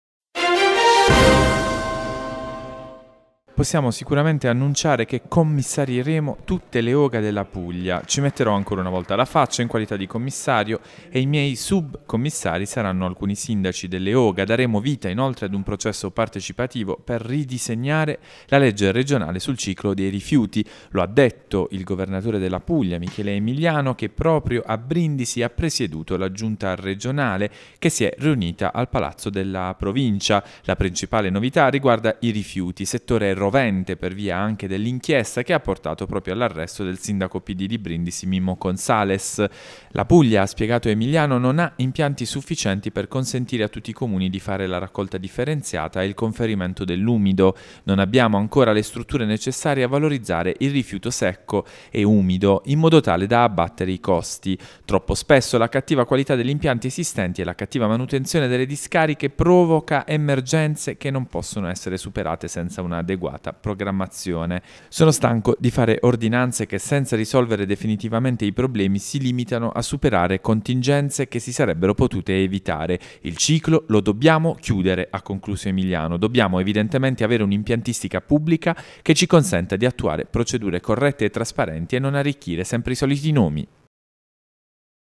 PUGLIA | Rifiuti, Emiliano: «Commissario le Oga»
Il Governatore di Puglia a margine della Giunta a Brindisi